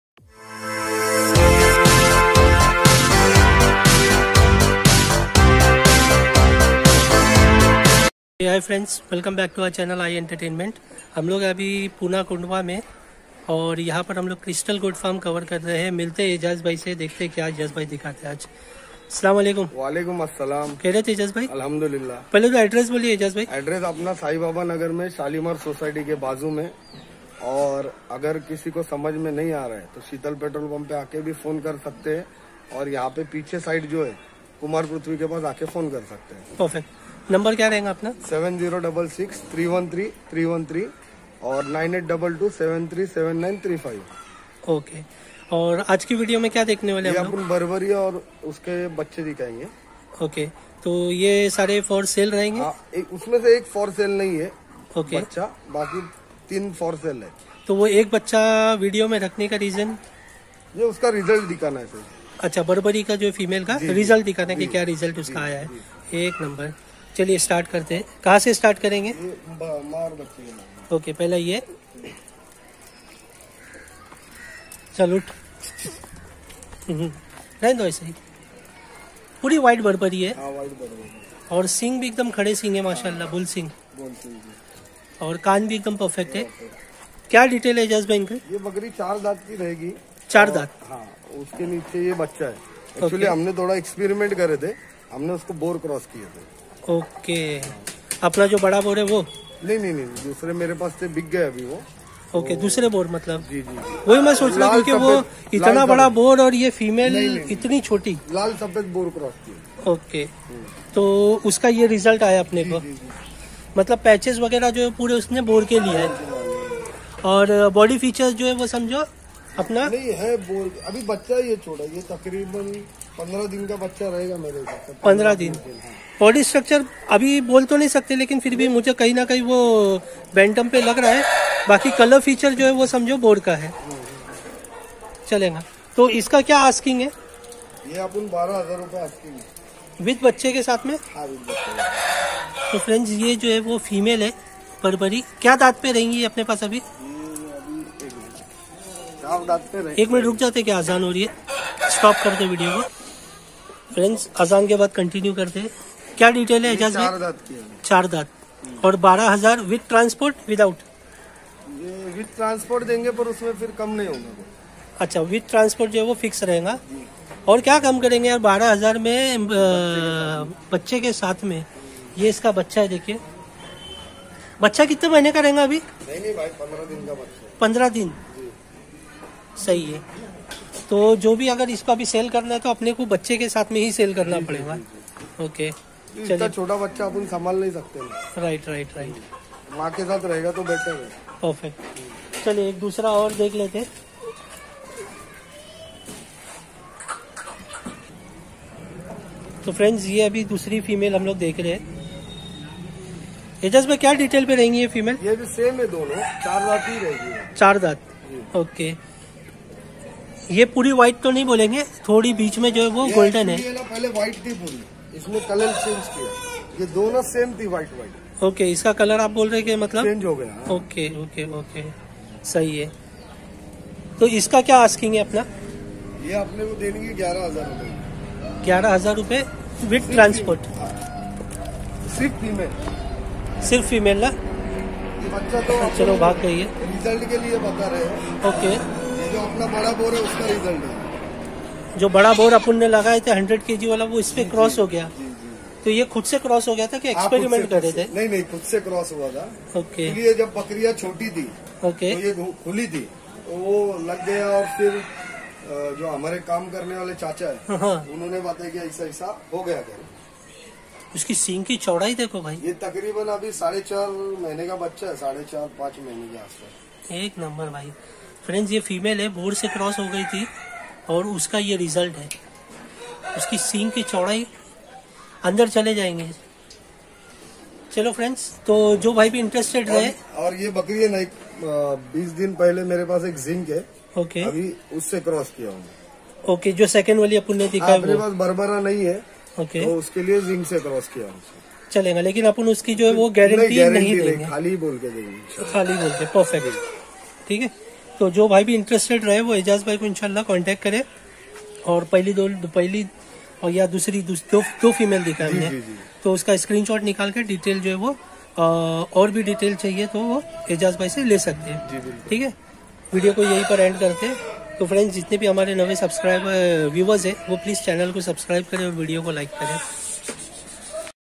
₹.12,000/- me Barbari Female with 1 Kid | Wo bhi Transport ke Sath | New Year Offer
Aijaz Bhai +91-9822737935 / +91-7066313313